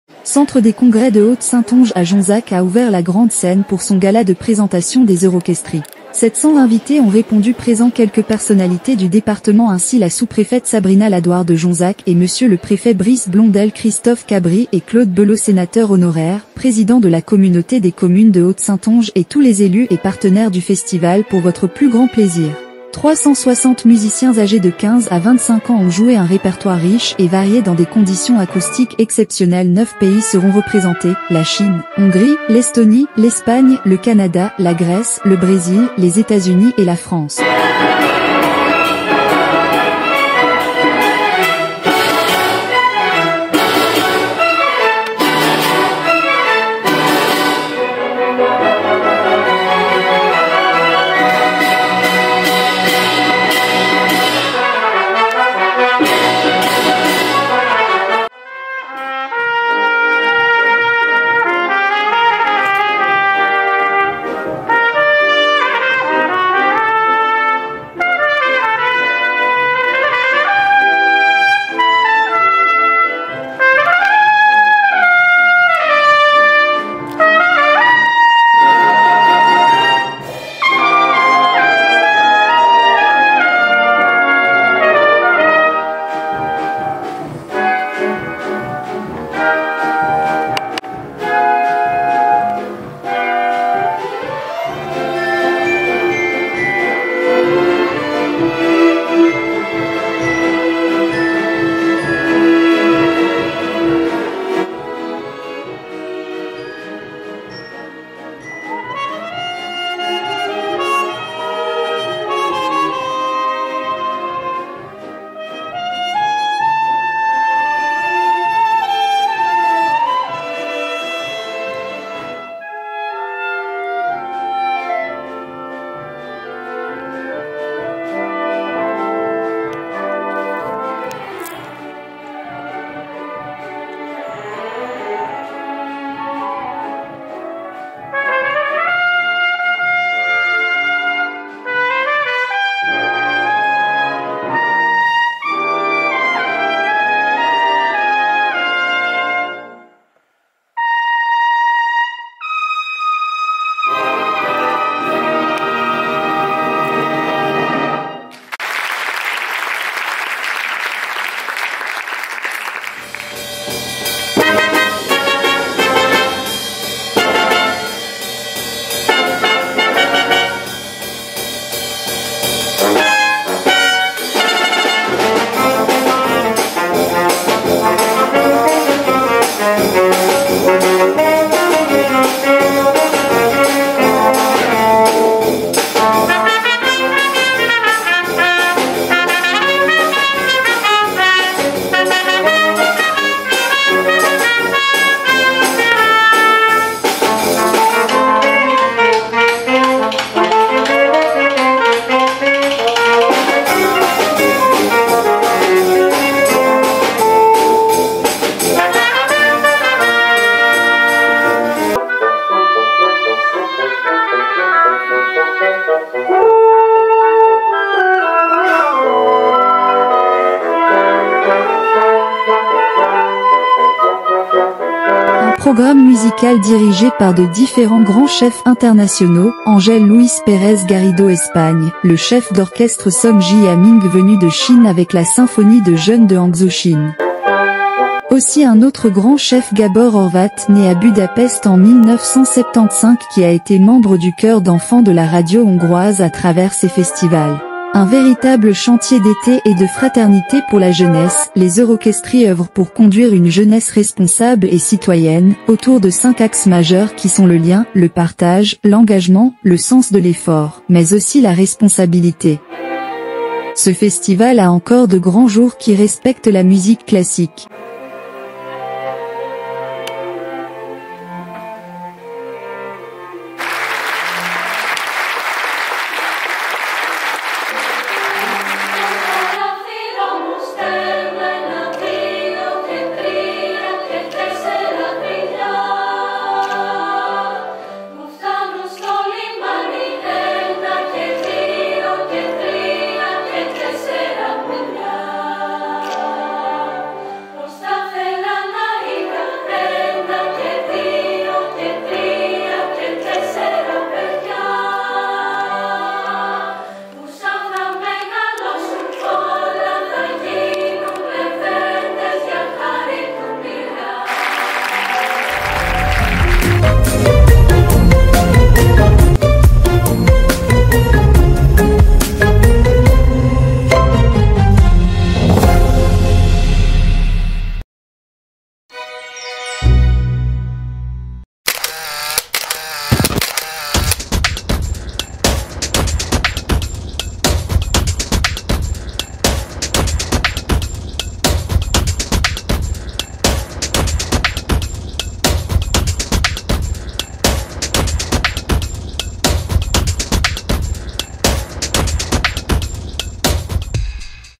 EUROCHESTRIES GALA D'OUVERTURE JONZAC 2024
Le centre des Congrès de Haute Saintonge à Jonzac à ouvert la grande scène pour son gala de présentation des Eurochestries. 700 invités ont répondu présents quelques personnalités du département ainsi la sous-préfète Sabrina LADOIRE de Jonzac et Monsieur le Préfet Brice Blondel Christophe Cabri et Claude Belot sénateur honoraire, président de la communauté des communes de Haute Saintonge et tous les élus et partenaire du festival.Pour votre plus grand plaisir, 360 musiciens âgés de 15 à 25 ans ont joué un répertoire riche et varié dans des conditions acoustique exceptionnel 9 pays seront représentés : la Chine, la Hongrie, l'Estonie, l'Espagne, le Canada, la Grèce, le Brésil, les États-Unis et la France. Un programme musical dirigé pars de différents grands chefs internationaux, angel luis Pérez Garrido Espagne , le chef d'orchestre SONG JIA MING venue de chine avec la symphonie de jeunes de HANGZHOU Chine.